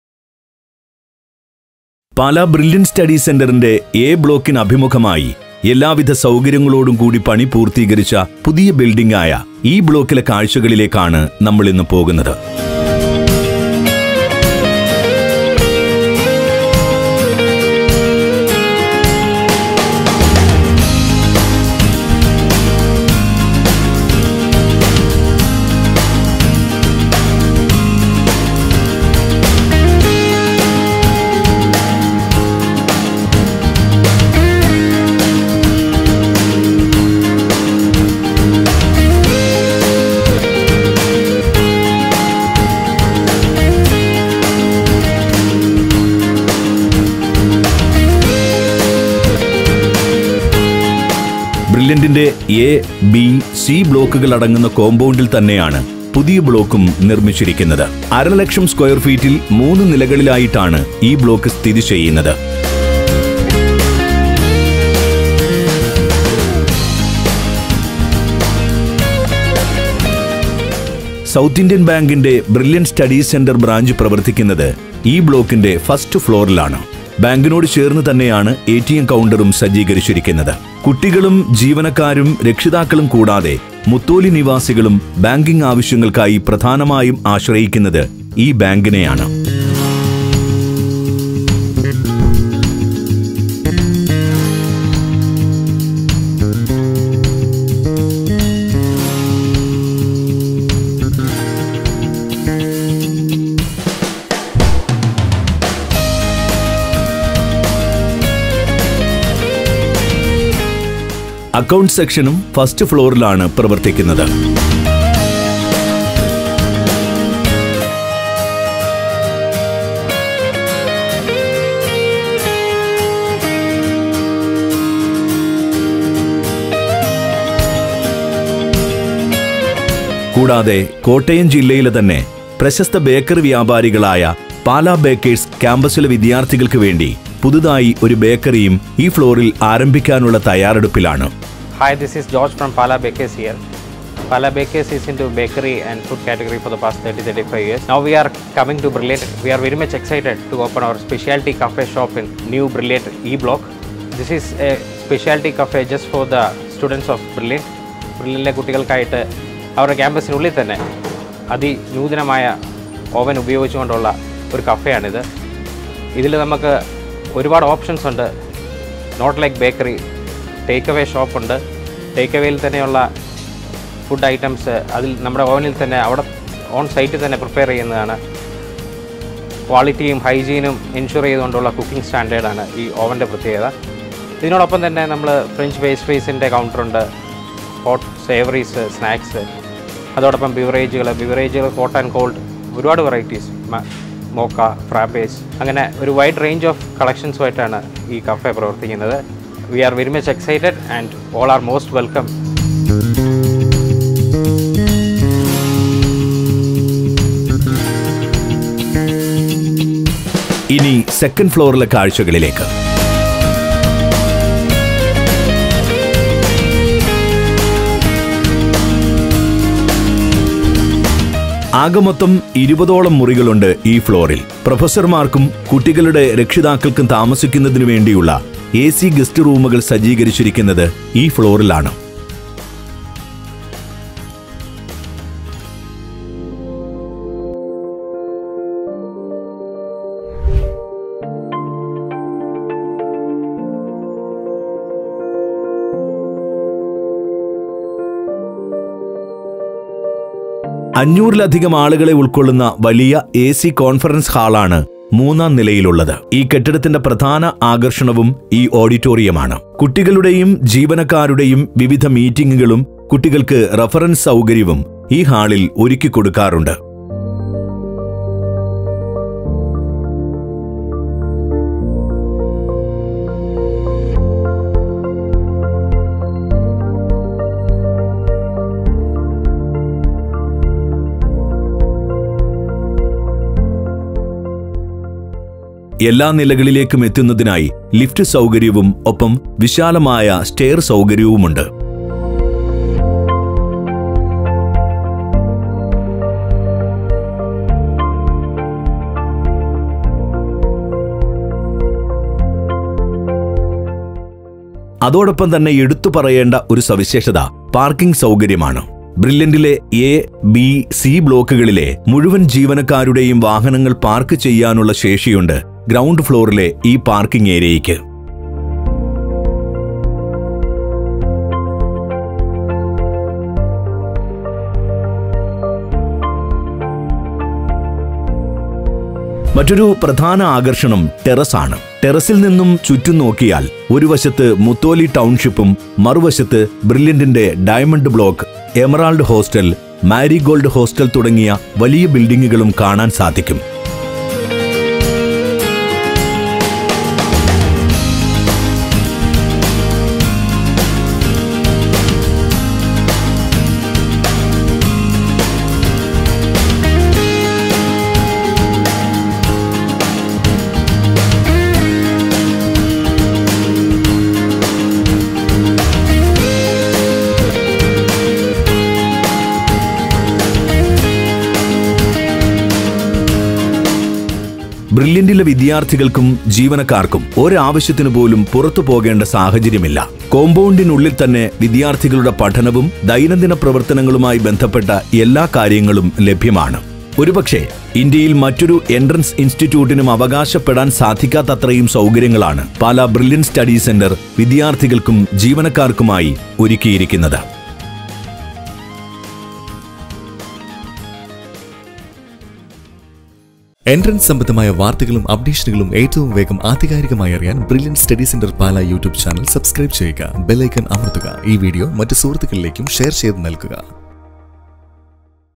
Brilliant Pala | Our New E Block
In this video, we are taking you through the new block on our main campus. The new building has a branch of SIB bank with an ATM counter, account session, cafeteria shop, AC guest room,  auditorium and spacious car parking area. The 3-story building is well equipped with a lift facility to move from one floor to another.

Come let's watch our new block.